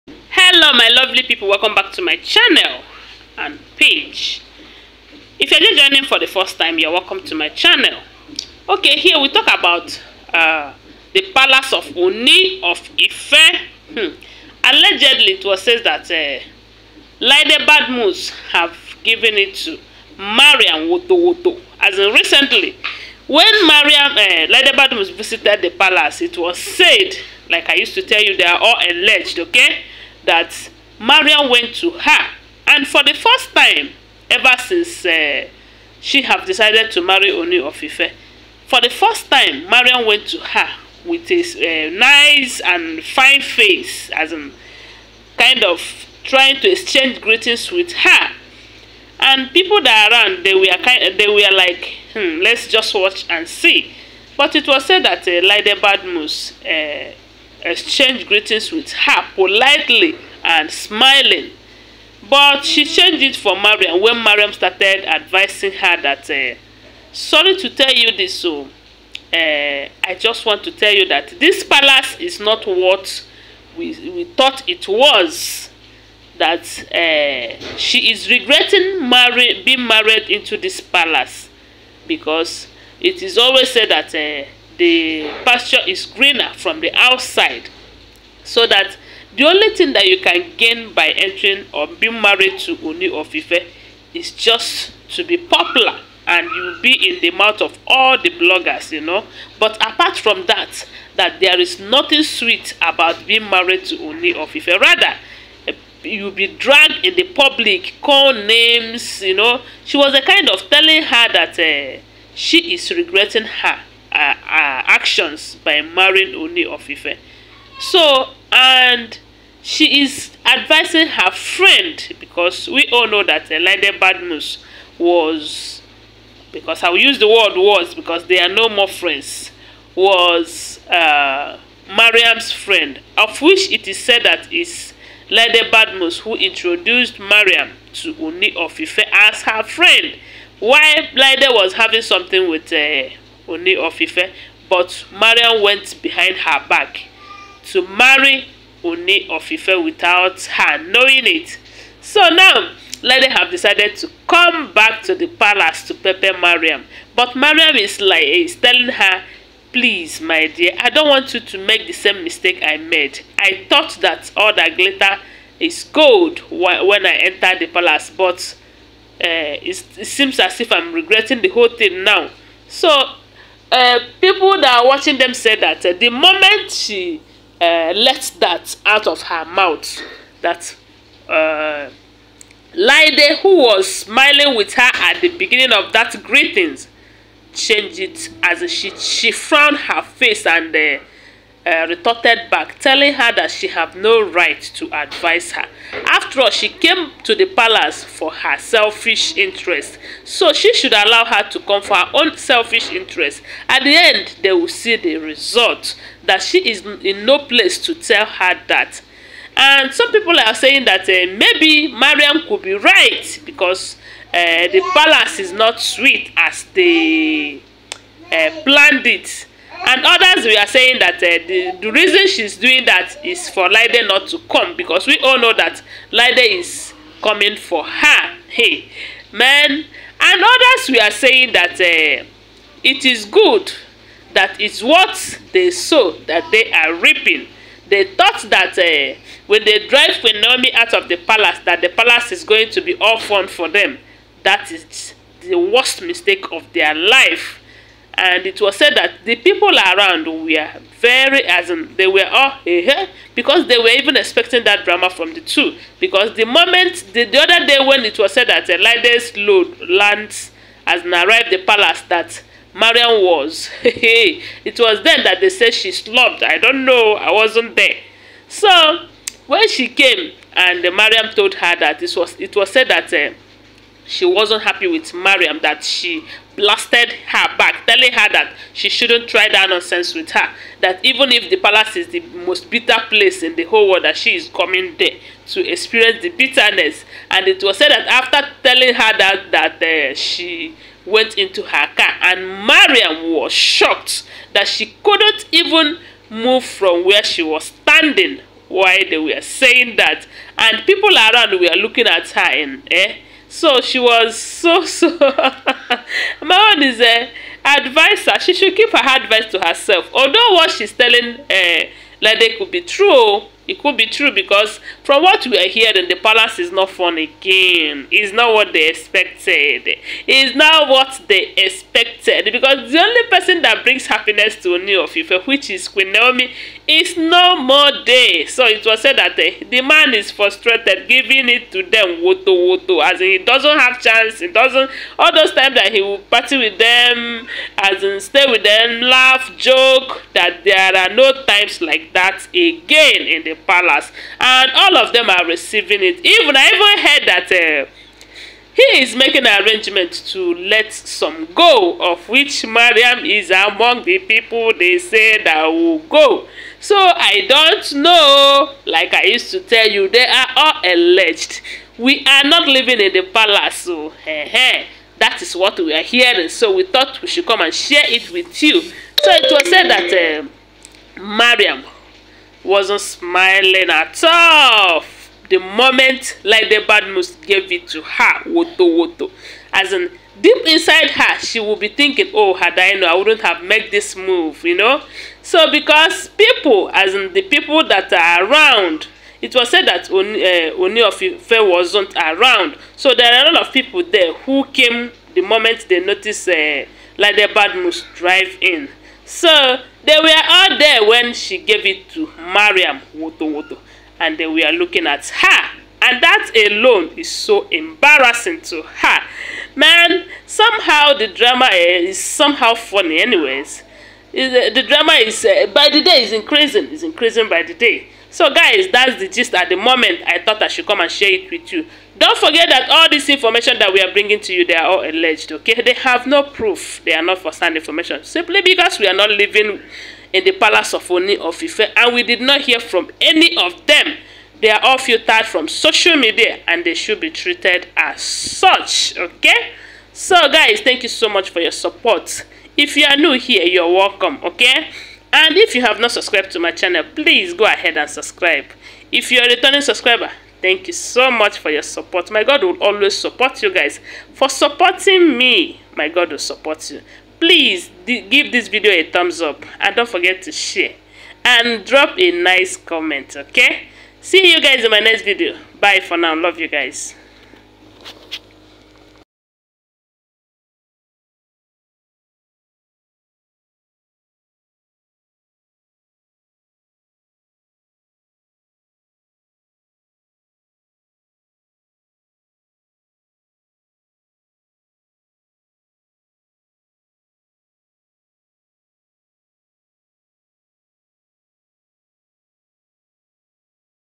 Ooni of Ife Shocked 😲 As laide Badmus Blasted Mariam. Queen Naomi 😯
Ooni of Ife Shocked 😲 As laide Badmus
Blasted Mariam. Queen Naomi 😯